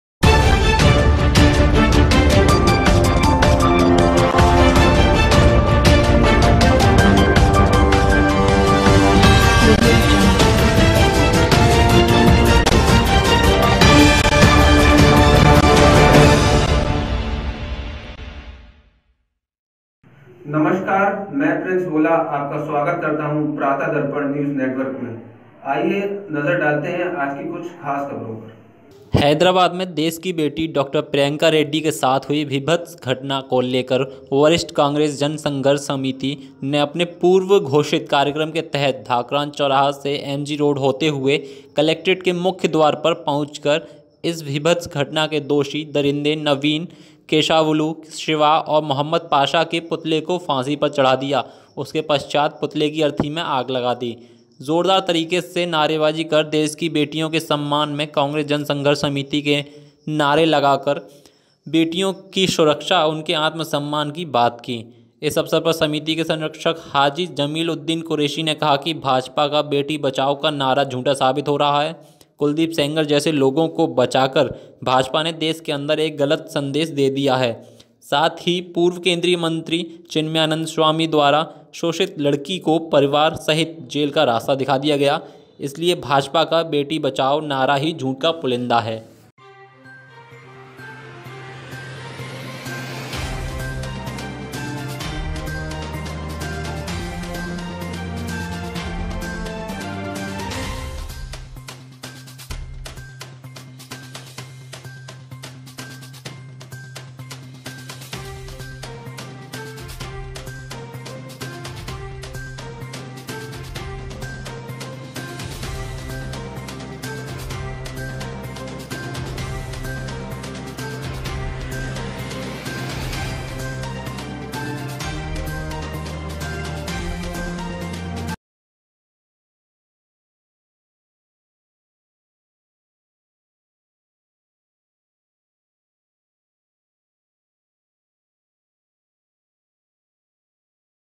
डॉ प्रियंका रेड्डी केस के आरोपी का फूंका पुतला
हैदराबाद में देश की बेटी डॉ प्रियंका रेड्डी के साथ हुई भीभत्स घटना को लेकर वरिष्ठ कांग्रेस जन संघर्ष समिति ने अपने पूर्व घोषित कार्यक्रम के तहत ढाकरान चौराहा से एमजी रोड होते हुए कलेक्ट्रेट के मुख्य द्वार पर पहुंचकर इस भीभत्स घटना के दोषी दरिंदे नवीन,केशावुलु,  शिवा,और मोहम्मद पाशा के पुतले को फांसी पर चढ़ा दिया, उसके पश्चात पुतले की अर्थी मे आग लगा दी।
 जोरदार तरीके से नारेबाजी कर देश की बेटियों के सम्मान में कांग्रेस जन संघर्ष समिति ने नारे लगाकर बेटियों की सुरक्षा उनके आत्मसम्मान की बात की, इस अवसर पर समिति के संरक्षक हाजी जमील उद्दीन कुरैशी ने कहा कि भाजपा का बेटी बचाओ का नारा झूठा साबित हो रहा है कुलदीप सेंगर जैसे लोगों को बचाकर भाजपा ने देश के अंदर एक गलत संदेश दे दिया है साथ ही पूर्व केंद्रीय मंत्री चिन्यानंद स्वामी द्वारा शोषित लड़की को परिवार सहित जेल का रास्ता दिखा दिया गया, इसलिए भाजपा का बेटी बचाओ नारा ही झूठ का पुलिंदा है।